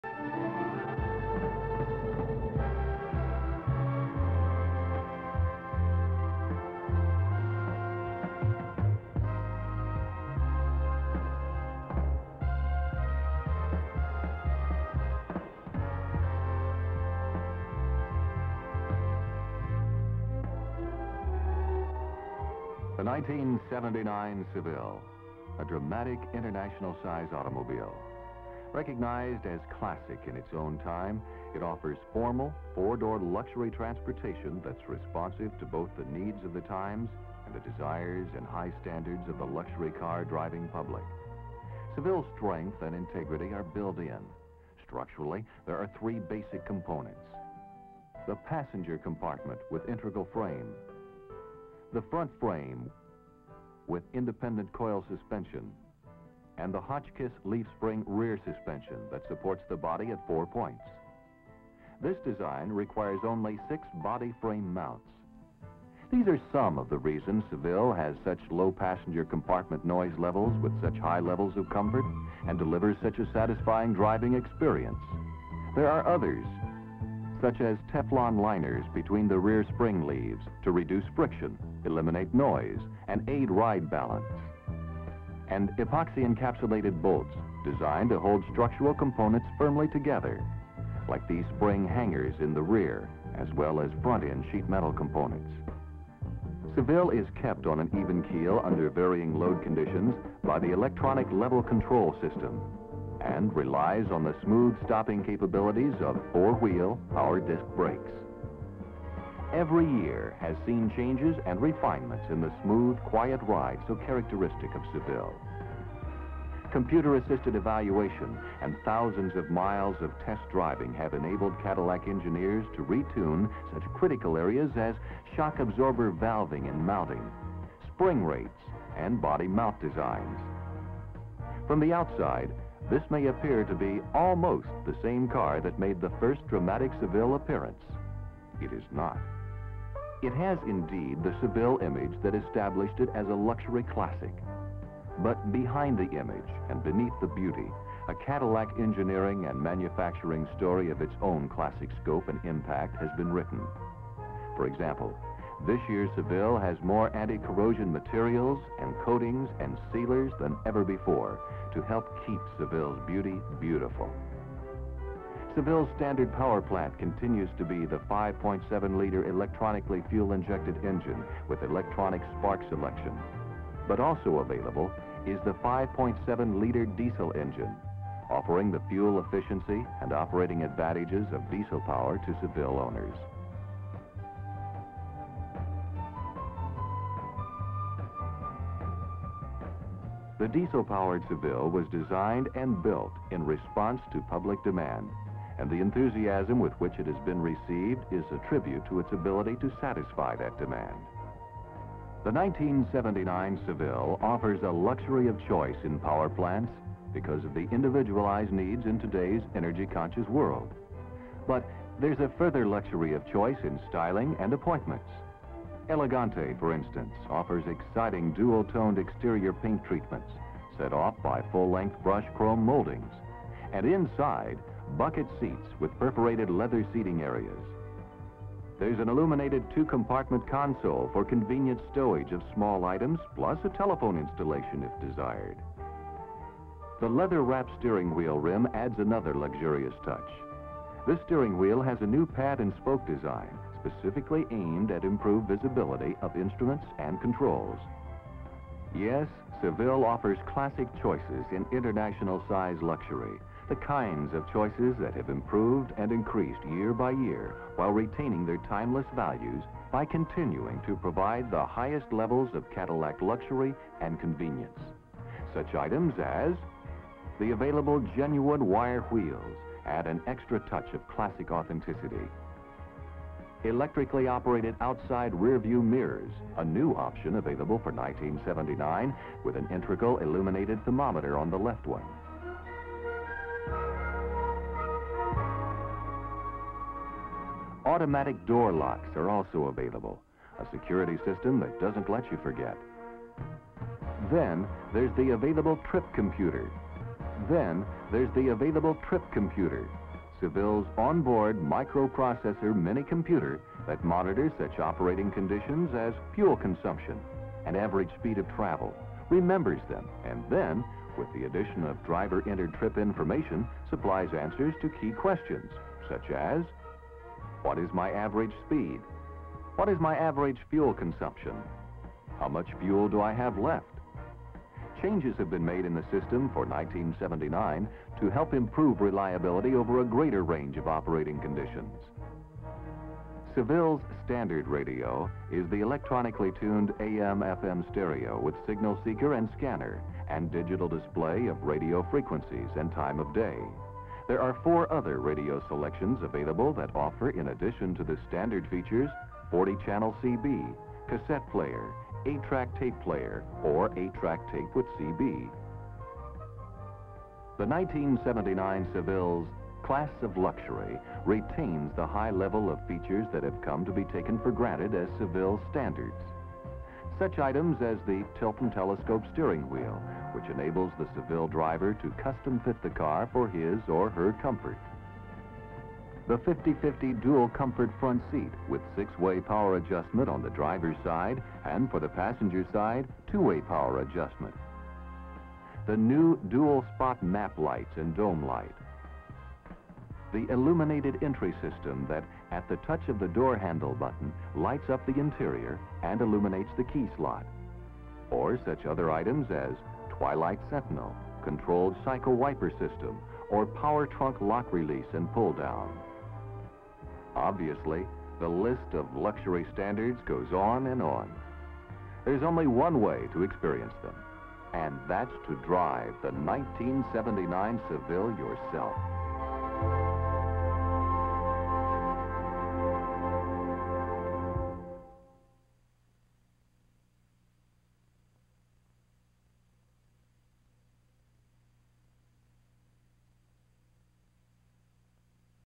1979 Cadillac Seville Intro
The Cadillac Seville is a luxury-type car that was manufactured by the Cadillac division of American automaker General Motors from 1975 to 2004, as a smaller-sized top-of-the-line Cadillac. The name of "Cadillac's first small car" was selected over a revival of LaSalle and the GM design staff's preference, LaScala, primarily because, notes GM Marketing Director Gordon Horsburgh, "It had no negatives. 1979 was the last year for this body style.

Ref: S072